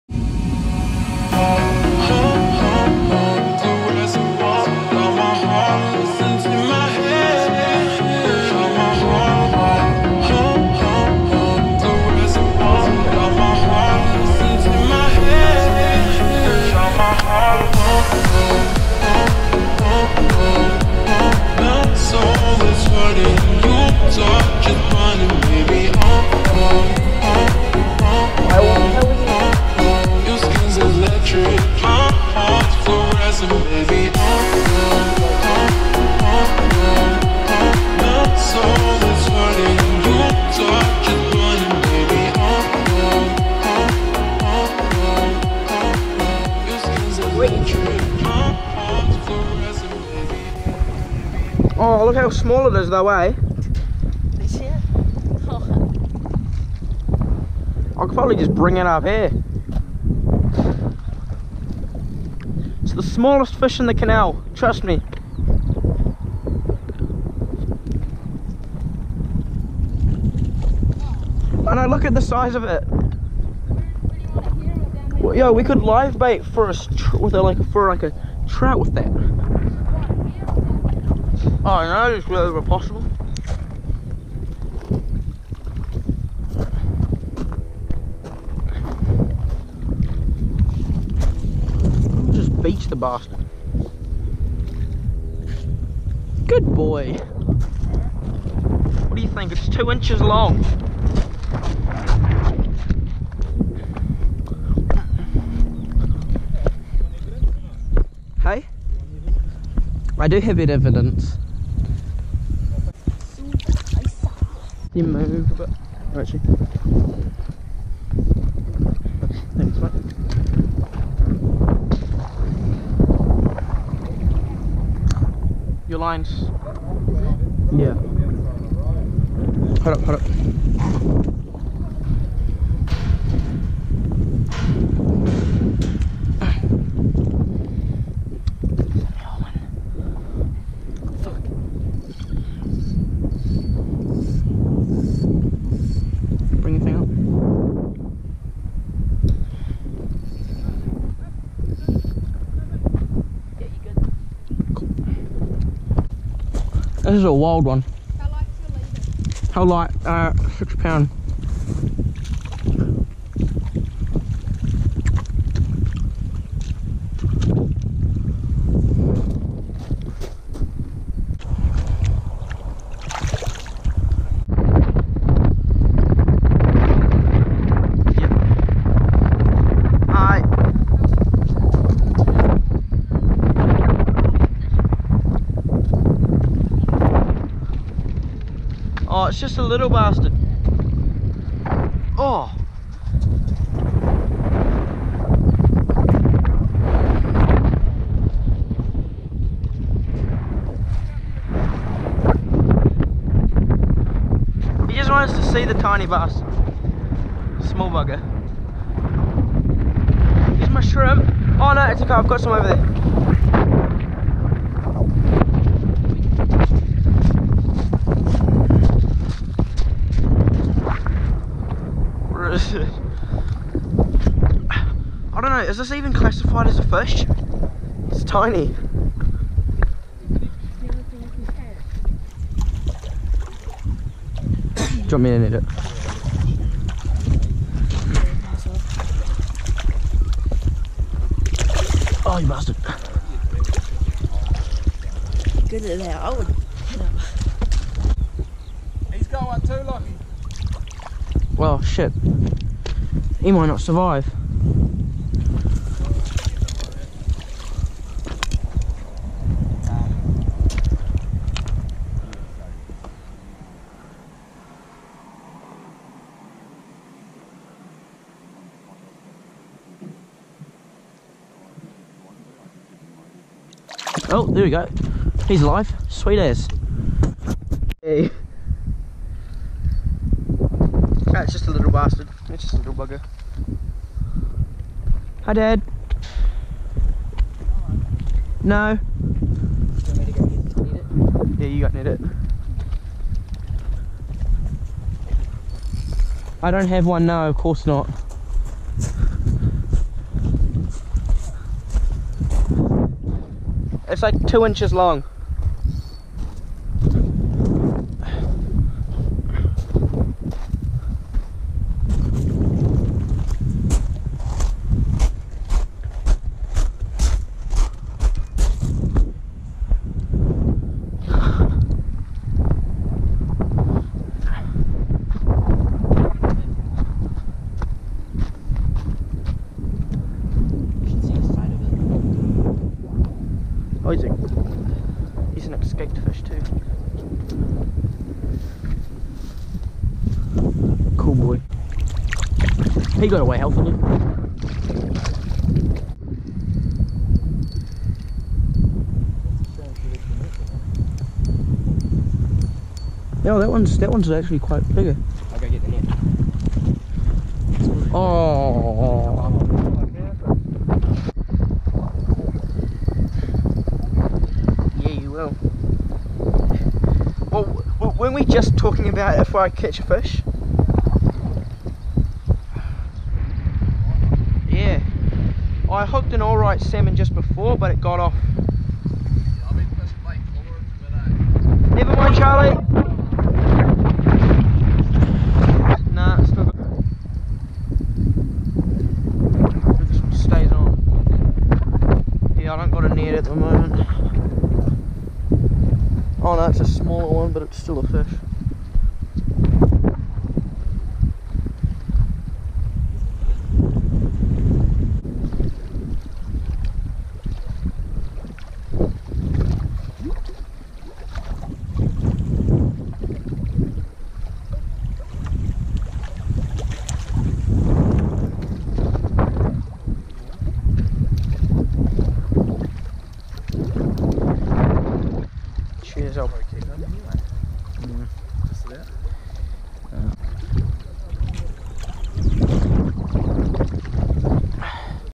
Salmon fishing | Twizel Ohau Canal
Just a few small salmon caught, fishing was very slow. Mate of mine managed to land a 11lb rainbow. Apart from that, enjoy and I have a perch fishing video coming soon. Cheers guys